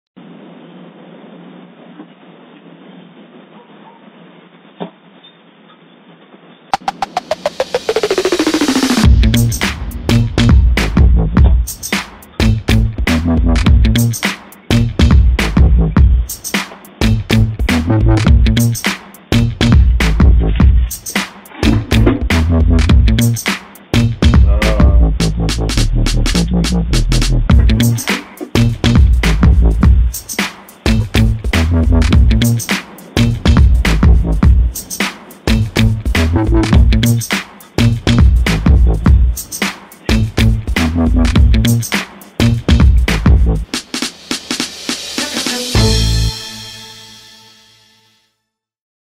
Mailman puts the 'pee' in USPS
Scott Anderson was horrified when he discovered footage of a postal worker urinating all over the porch of his house in Memphis, Tennessee. The local USPS office issued an apology to Anderson and has opened an investigation into the incident.